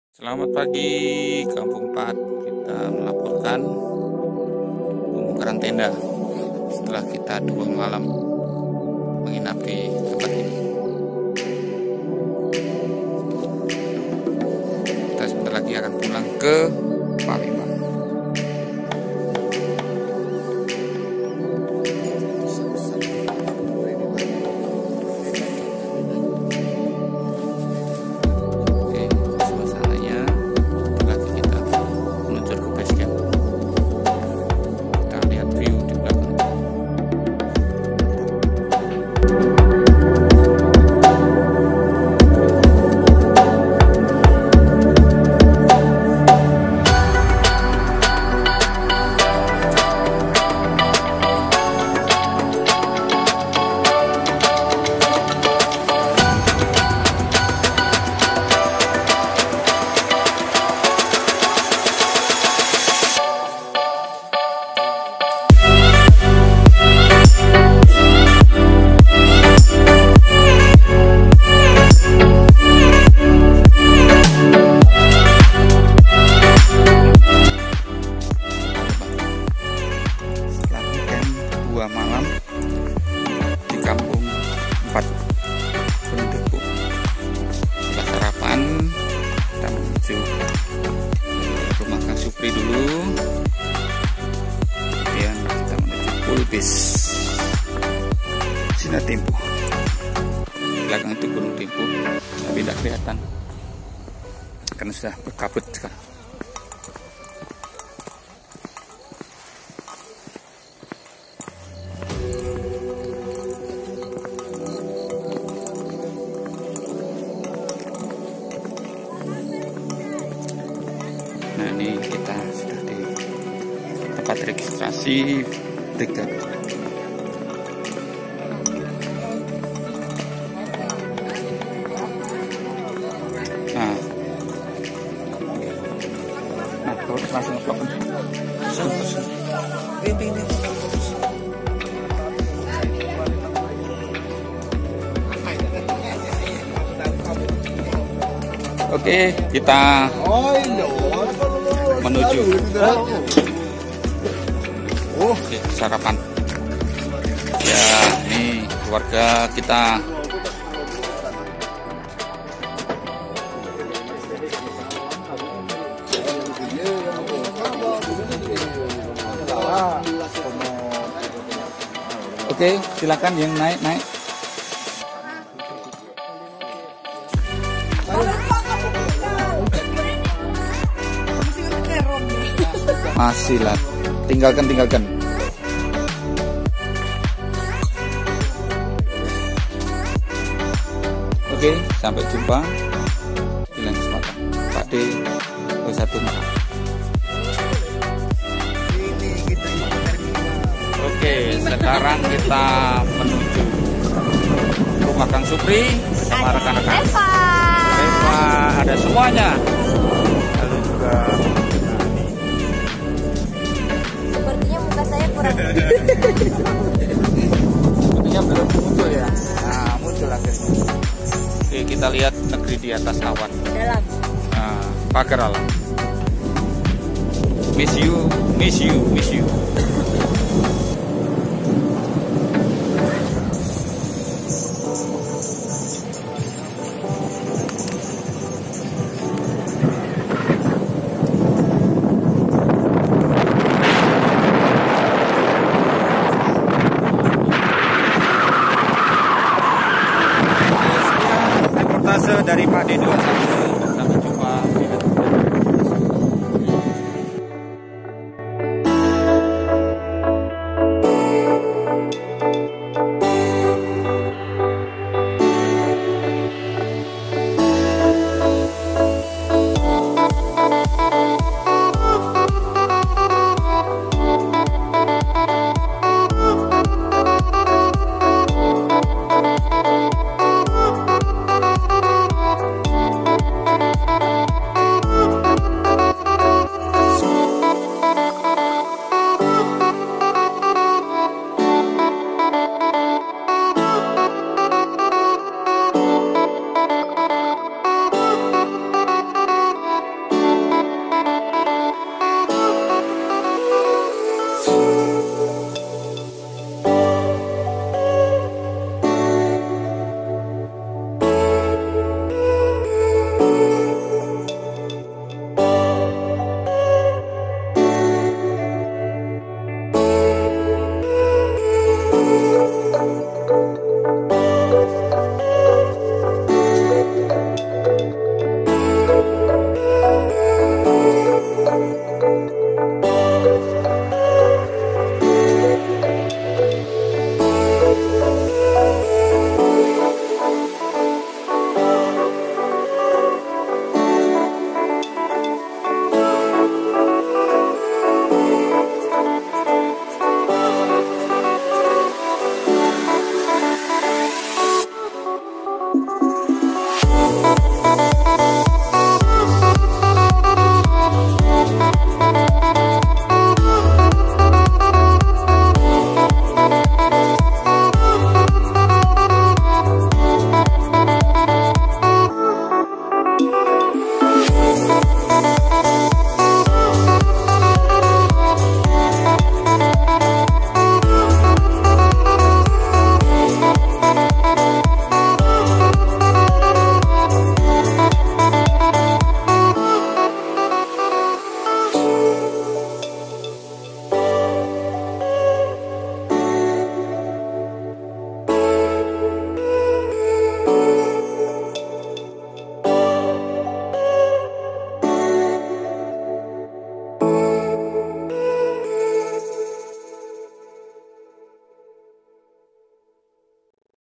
Camping Ceria Kampung IV Dempo I Day 3 - See u again
See u again. Ada perjumpaan adan perpisahan. Tak terasa 3 hari 2 malam bercamping ceria di Kampung IV Dempo berakhir sudah. Terima kasih  Tuhan atas berkat dan pernyertaan-Mu, kami semua bisa melawatinya dengan aman dan diberi cuaca yang cerah serta kesehatan. Terima kasih buat keluarga, rekan-rekan pendaki, Brigade, Warung Noval,Kang Supri, Pak RW, Wawan Gondres,  Tyas Black, Sandi, Bayu, etc.  Salam Lestari.